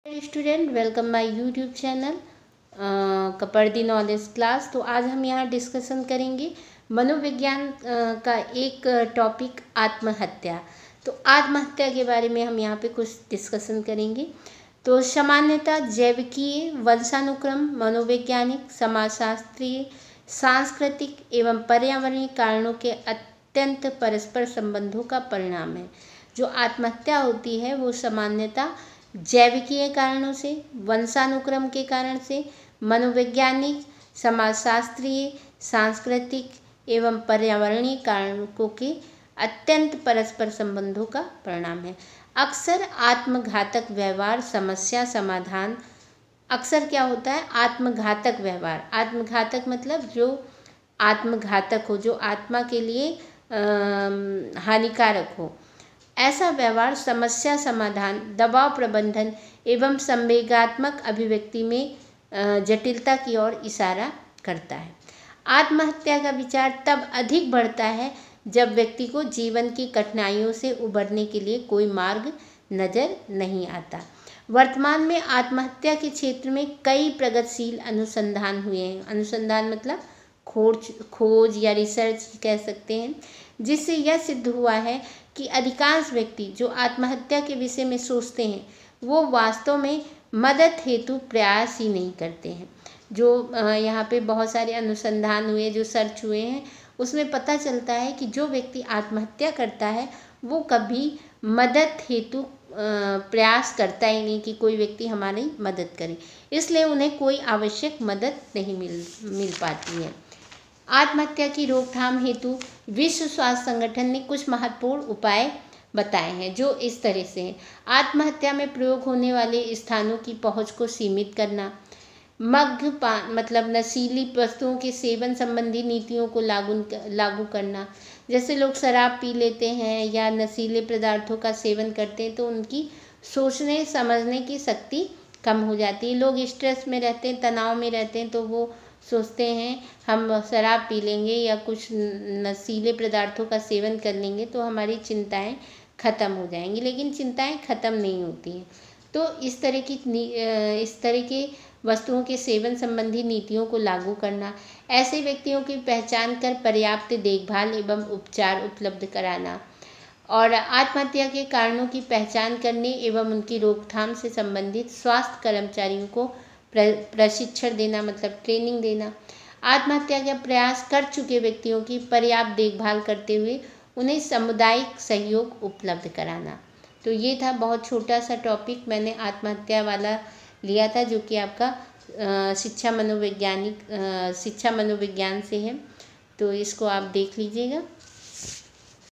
12th education exam 2024/शिक्षा शास्त्र आत्महत्या l#monishuklamaam
इस वीडियो में मैं शिक्षा मनोविज्ञान का टॉपिक आत्महत्या से रिलेटेड चर्चा की